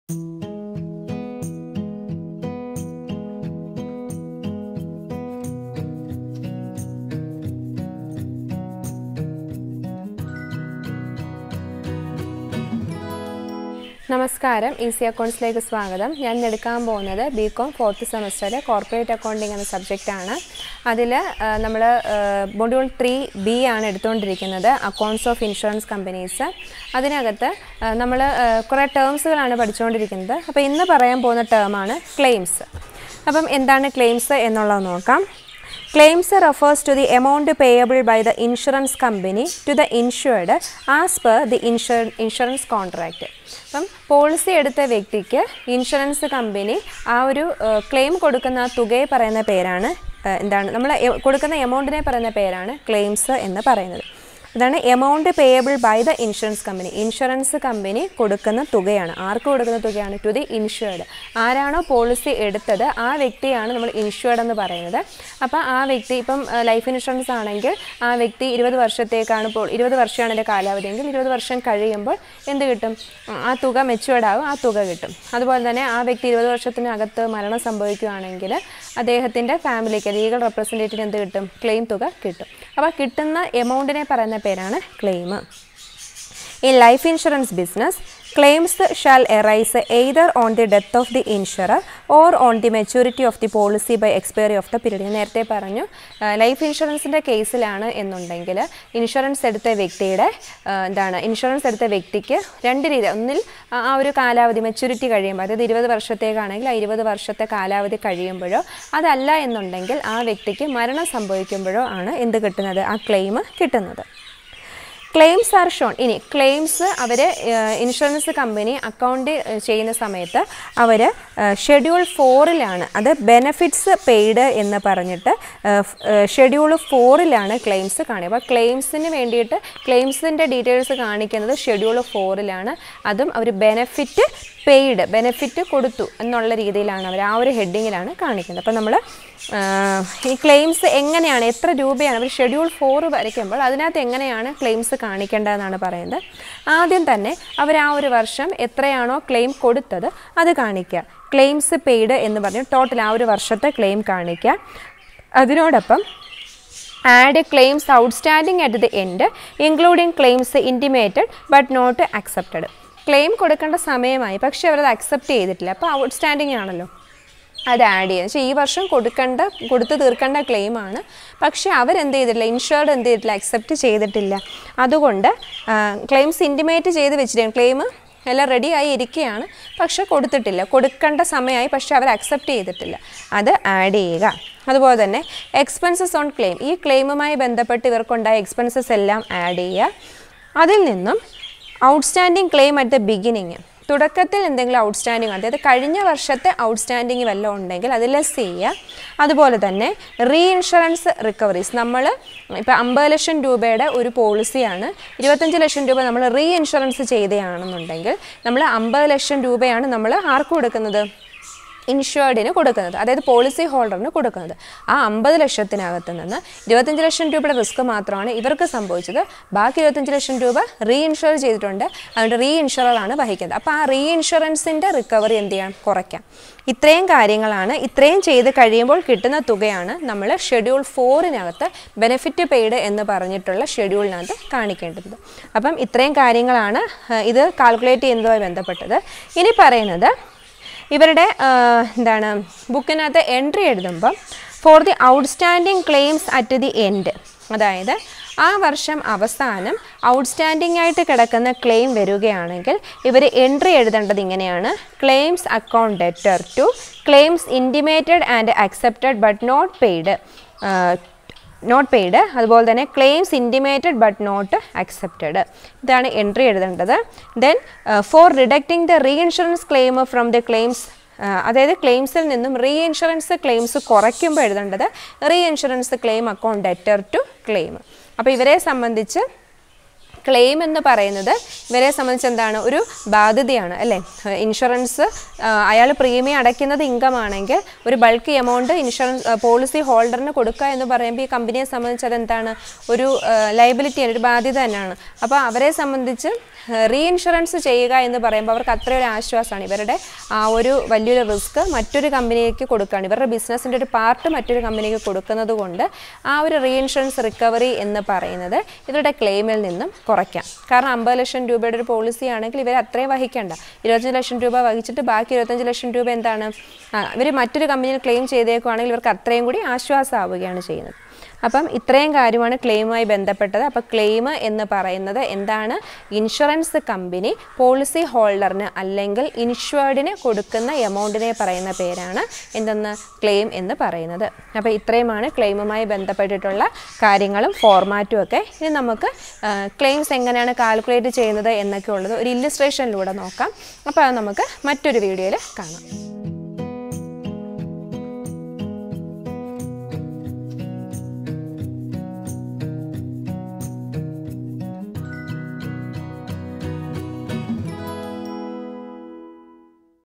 Degree 4th Sem | Corporate Accounting | Module-3(B) | Accounts Of Insurance Companies | (Part-08)
Degree 4th Semester | Corporate Accounting | Module-3(B)-(Part-08) | Accounts Of Insurance Companies | Term's in Insurance (Part-04) | Claims | Easy Accounts |

Class          :Degree 4th Semester
Subject       : Corporate Accounting
Module       :3(B) (Part-08)
Chapter      :Accounts Of Insurance Companies
Topic          : Term's in Insurance (Part-04)
Sub Topic  : Claim's

By,
Easy Accounts